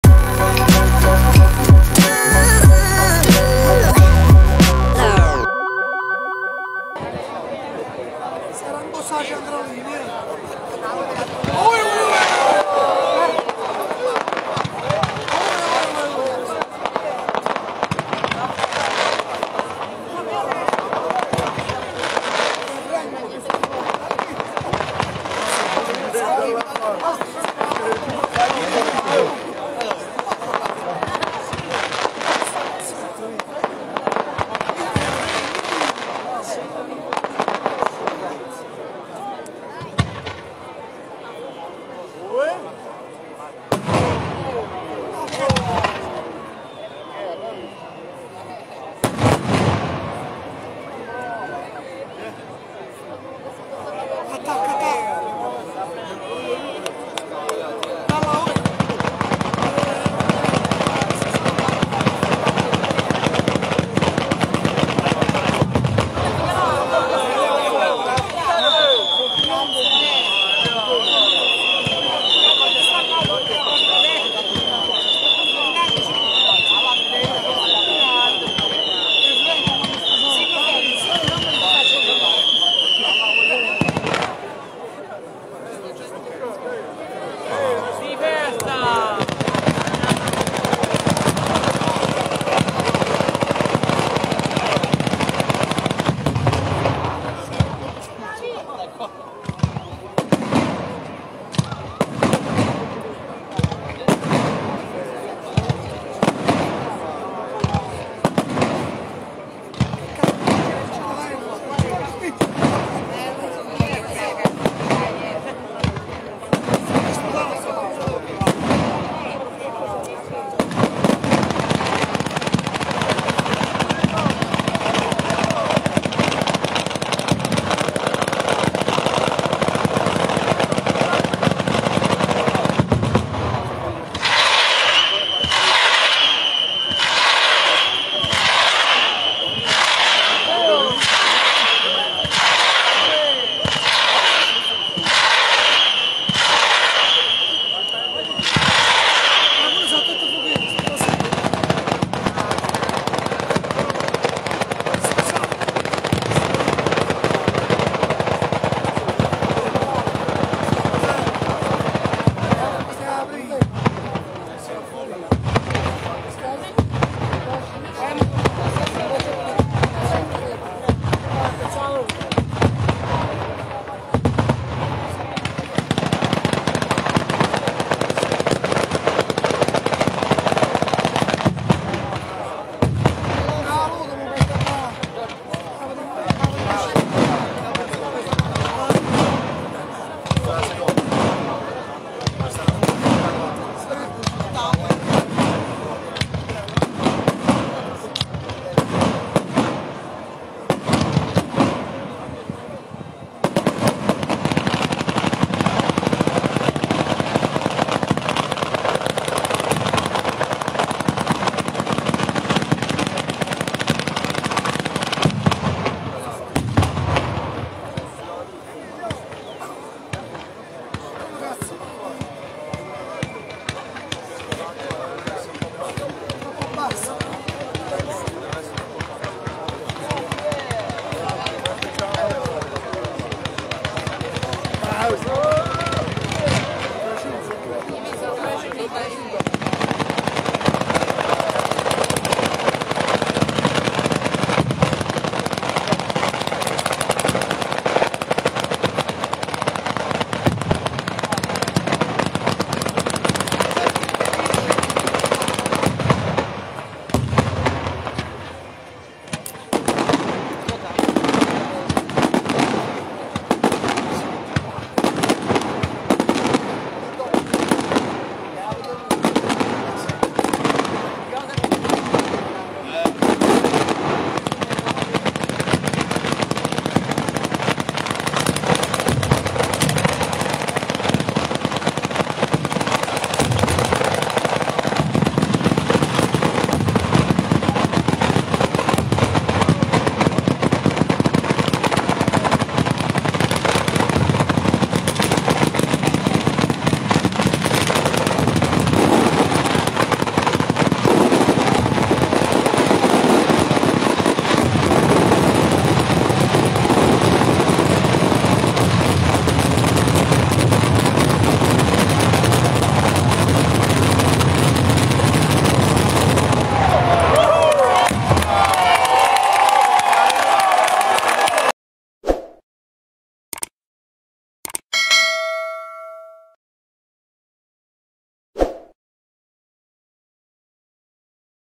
Spettacolo pirotecnico in onore di San Sabino Vescovo 2022 - Torremaggiore (Fg) 28 giugno 2022
Torremaggiore 28 giugno 2022 Spettacolo  pirotecnico nella serata conclusiva dei festeggiamenti  in onore di San Sabino Vescovo 2022 in Torremaggiore (Fg) a cura della ditta  - Nuova Pirodaunia -
Video dalla pagina Facebook: @eventiamatoriali in collaborazione con @apulianblogger e @AntonioFania
Seguici anche su Facebook al seguente indirizzo: @piroviaggiando